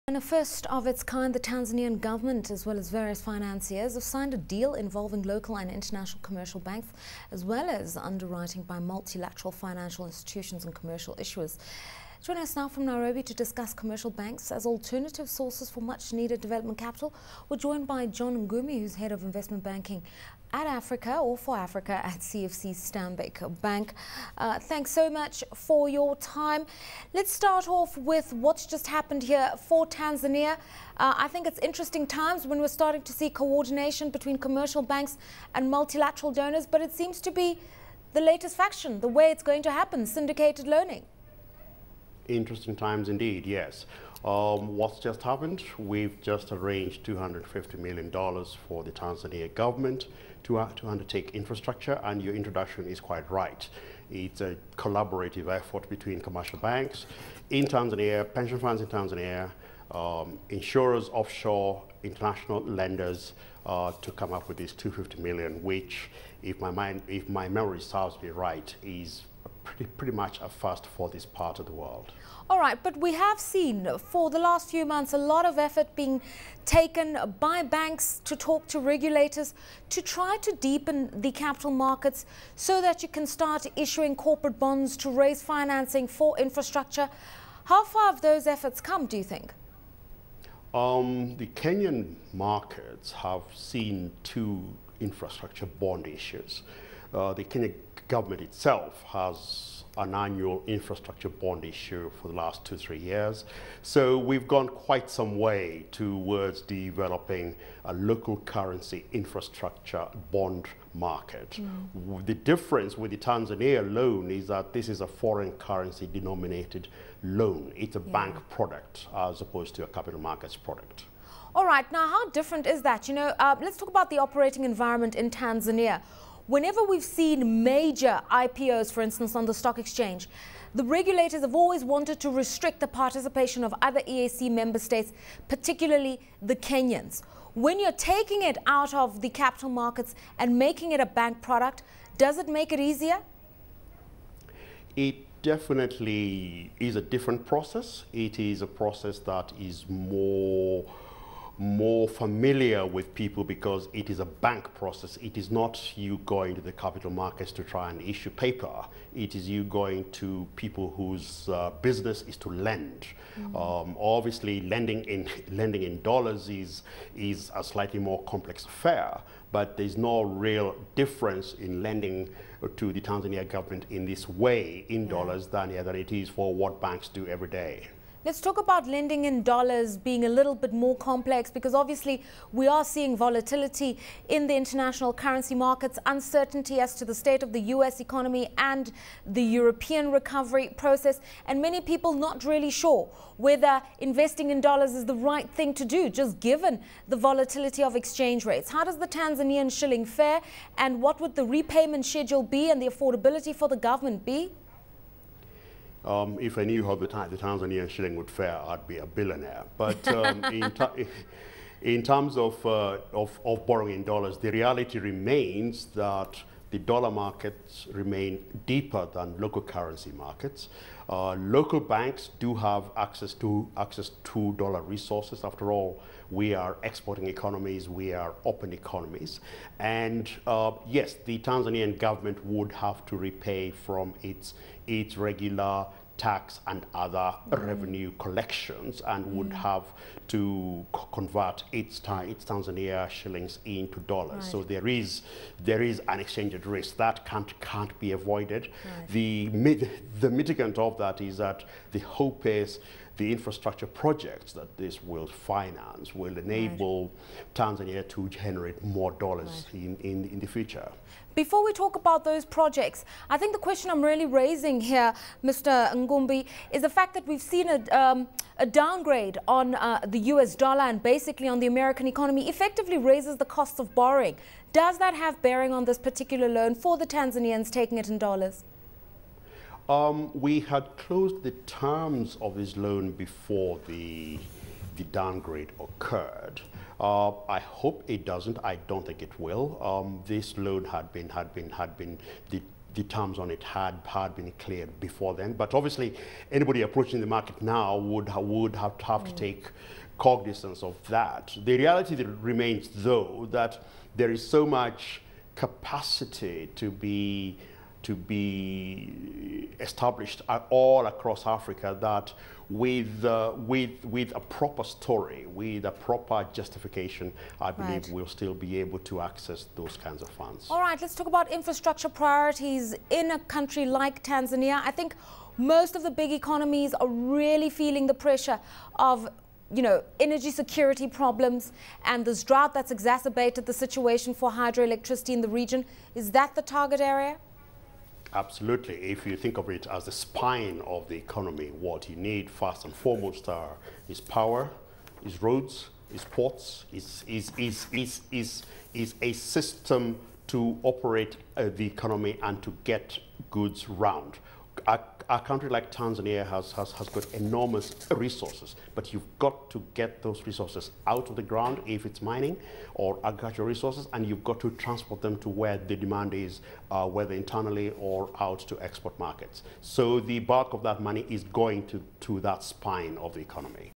Sourcing Development Capital from Tanzania's Commercial Banks
The Tanzanian government and various financiers signed a deal involving local and international commercial banks as well underwriting by multilateral financial institutions and commercial insurers. Joining ABN's Lerato Mbele from Nairobi to discuss commercial banks as alternative sources for much-needed development capital is John Ngumi, Head of Investment Banking for Africa at CFC Stanbic Bank.